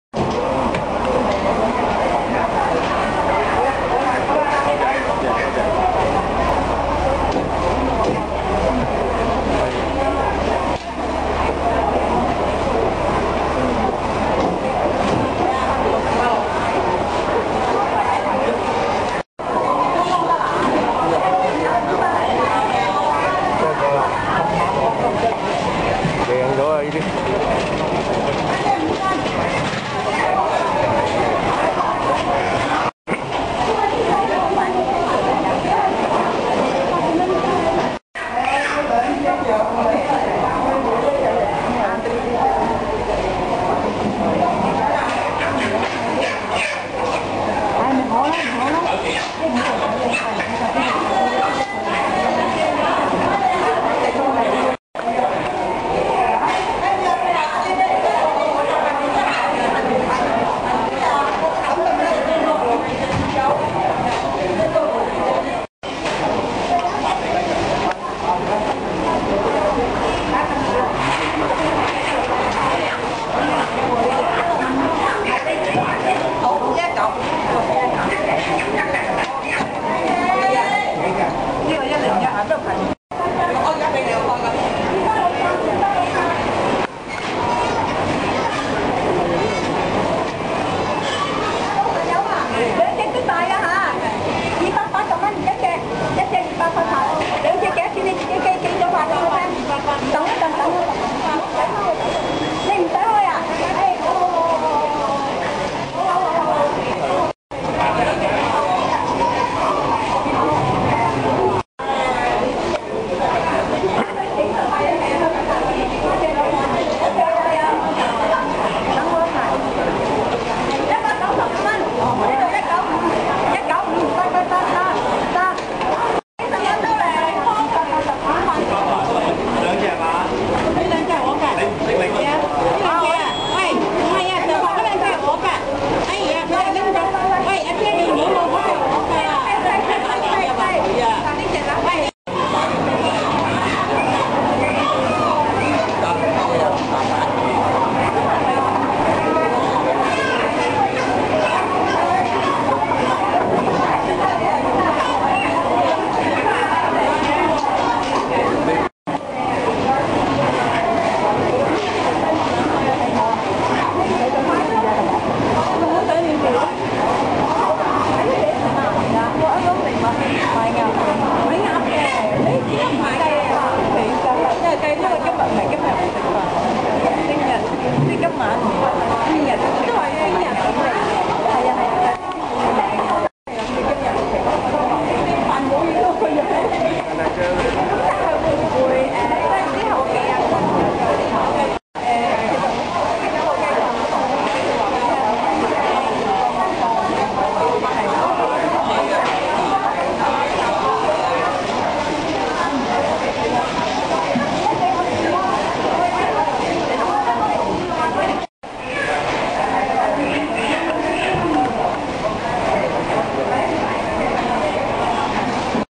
除夕雞情直擊報導 Chicken selling reported in the early morning eve of Lunar New Year
鼠年大除夕 街市雞情直擊報導
People buying chicken to celebrate the Lunar New Year's.